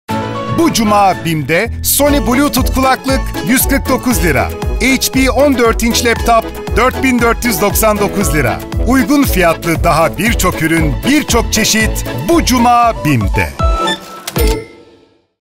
BİM - Sony Bluetooth Kulaklık, HP Laptop
Bu Cuma BİM’de Sony Bluetooth Kulaklık 149 TL, HP 14 inç Laptop 4.499 TL Uygun fiyatlı daha birçok ürün birçok çeşit bu Cuma BİM’de!